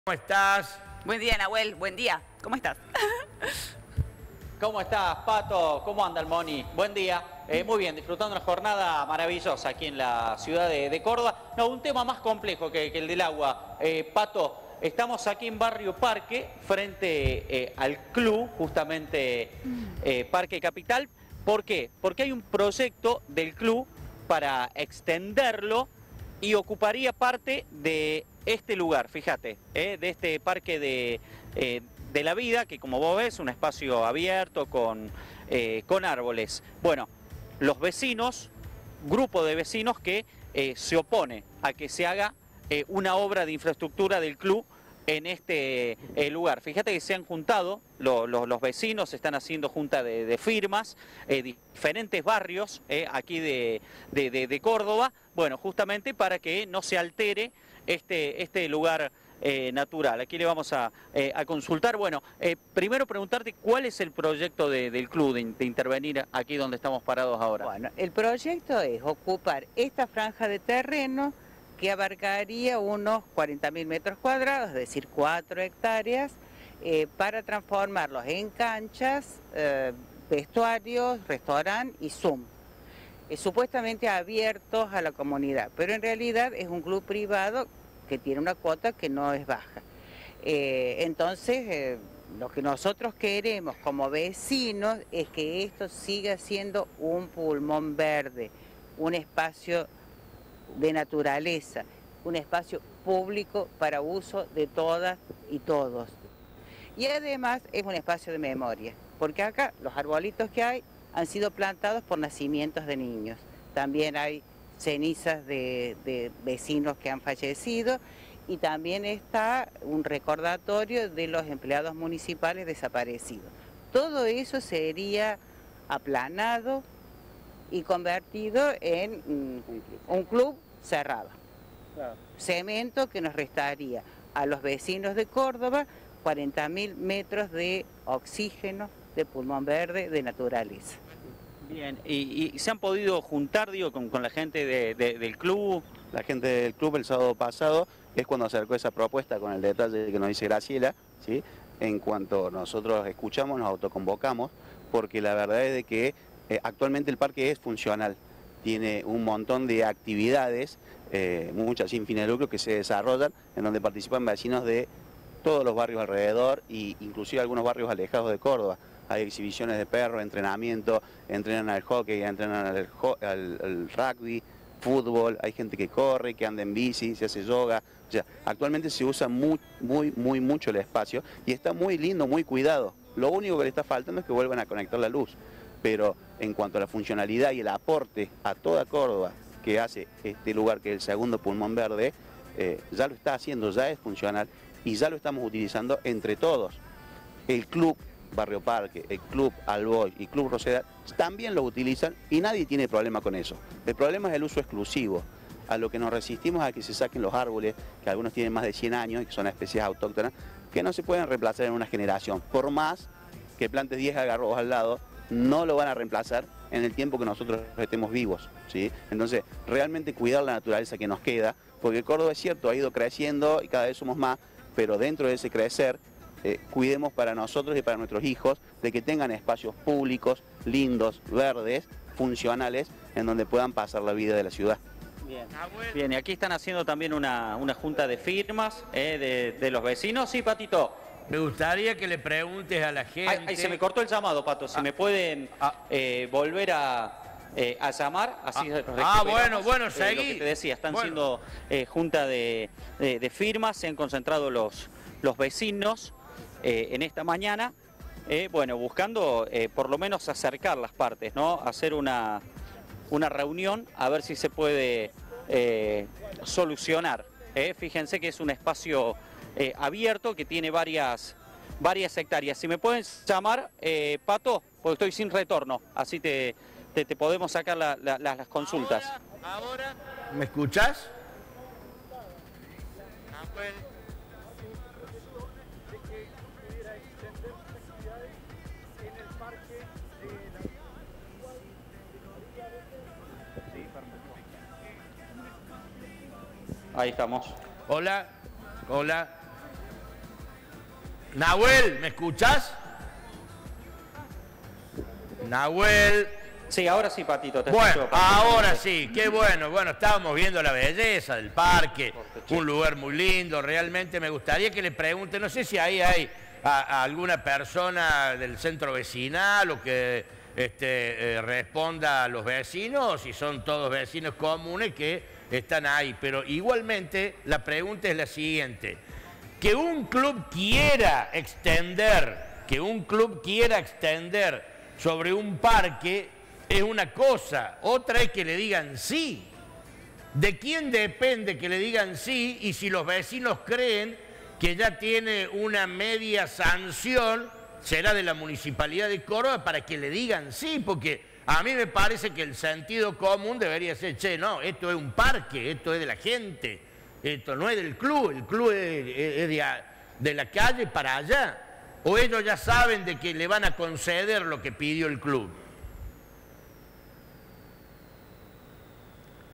Vecinos se exponen a que el Club se extienda sobre el espacio verde.
Móvil para Pato a la mañana.